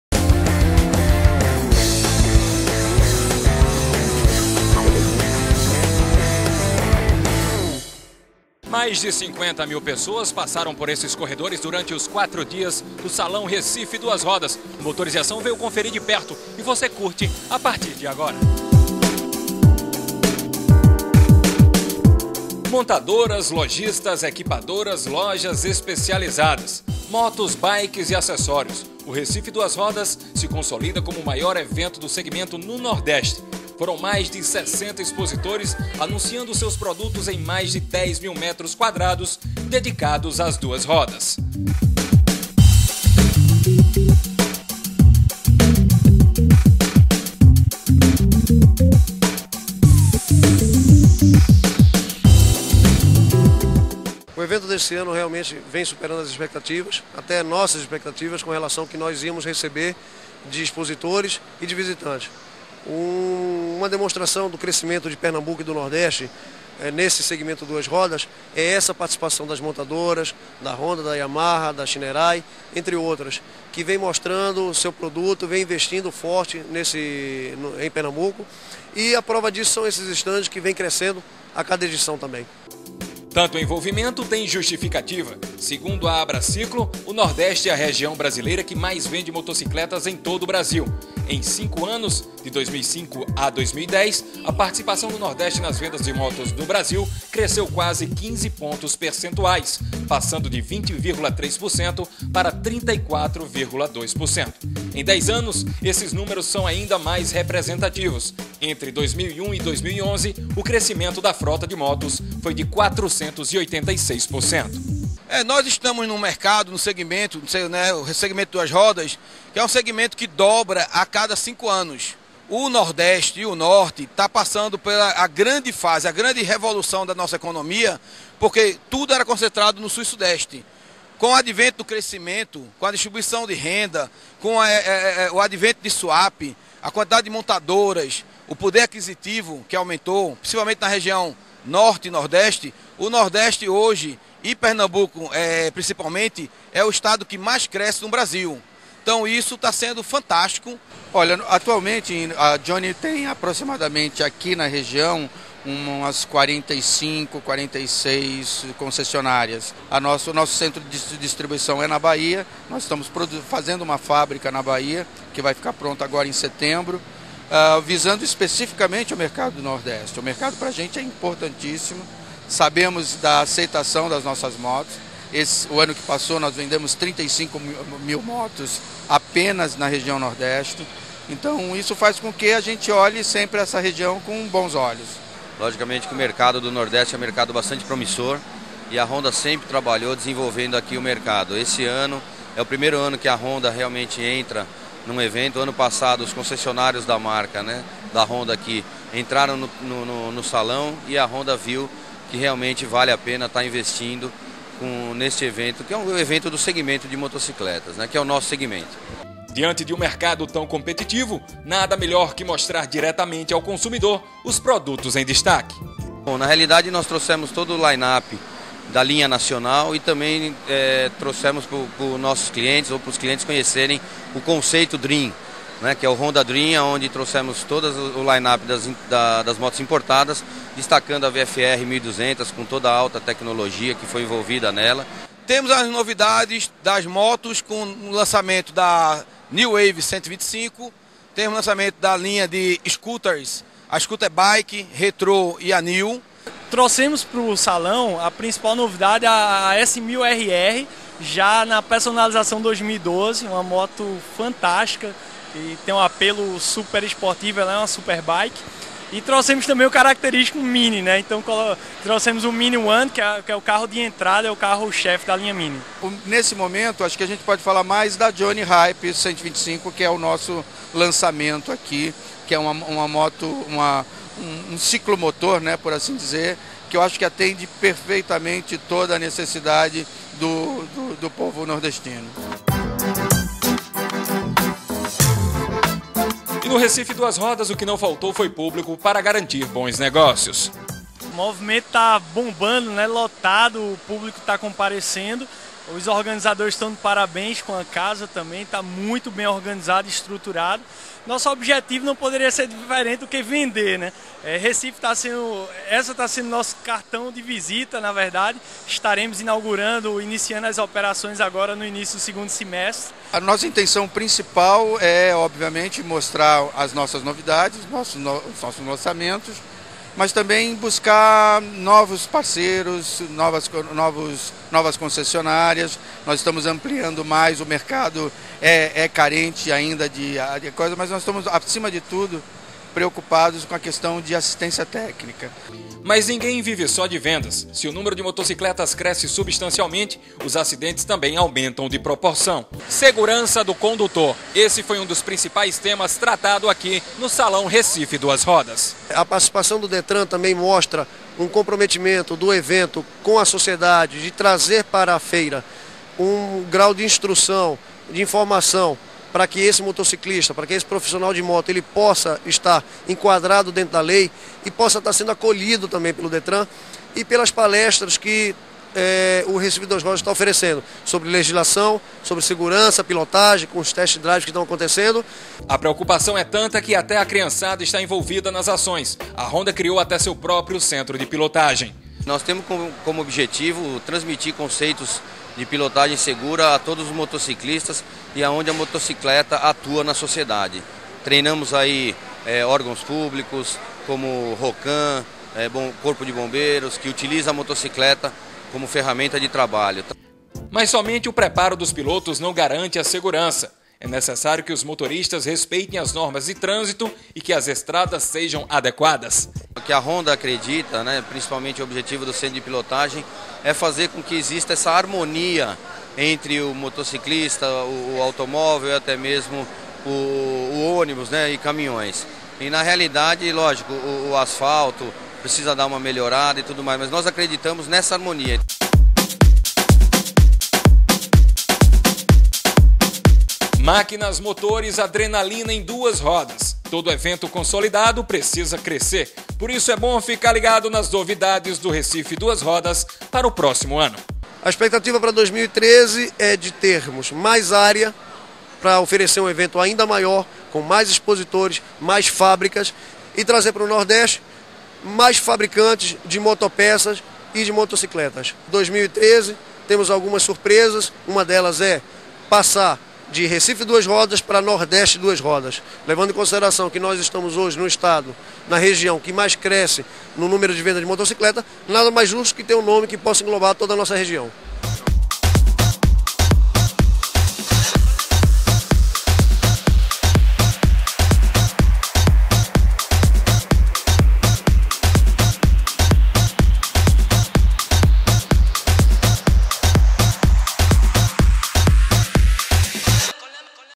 Recife Duas Rodas 2012
O @motoreseacao marcou presença na edição 2012 do Salão Recife Duas Rodas. Acompanhe reportagem especial.